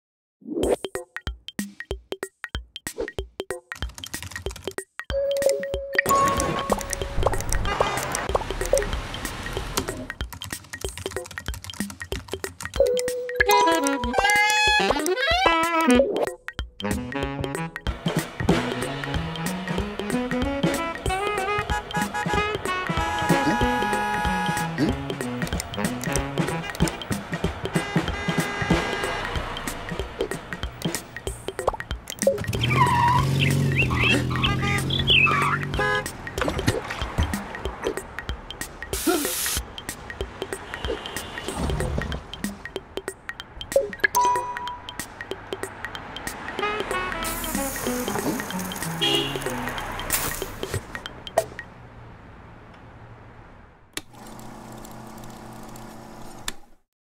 Who's Afraid of AI Art?
A (very) short film about an artist going on strike against the scary new world of AI-generated art.

I created this film using traditional hand drawn images together with AI-generated images*, as an experiment to see if human artists and AI can work together in harmony... or not.

*AI image generators like Dall-E are artificial intelligence that generates original works of art based on human-supplied prompts.

Credits:
Directed and animated by Yuval Haker
Additional animation and sound design by Dana Roth